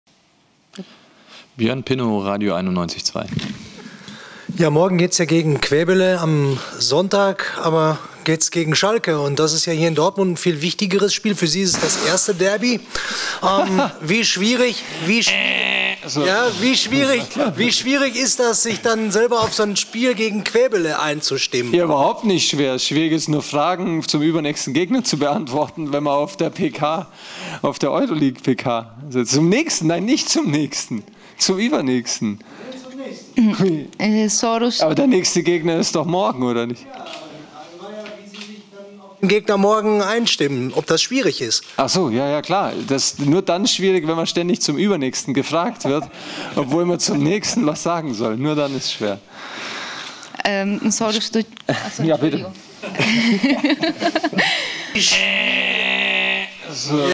Ja, ist denn heute schon Schalke ??
Dortmund, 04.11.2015: Während der Pressekonferenz wurde Cheftrainer Thomas Tuchel schon auf das Revierderby gegen den S04 angesprochen. Seine Antwort war recht ausgeprägt :)